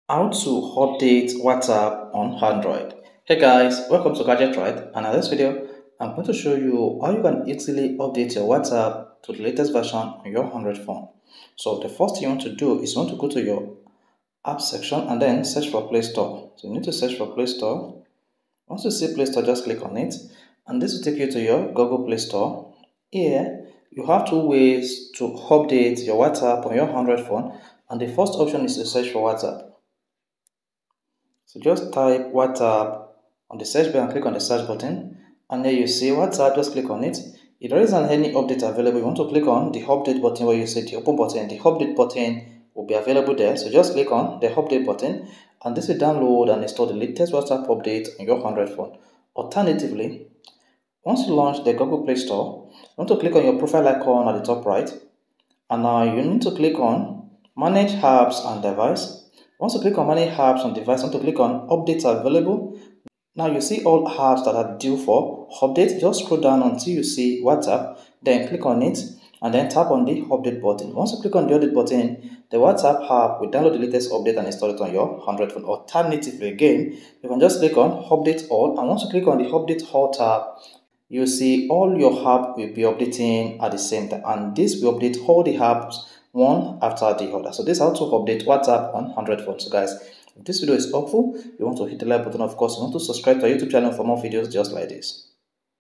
How to Update WhatsApp to the Latest Version
In this video, you will learn how to update your WhatsApp to the latest version on your Android and iPhone.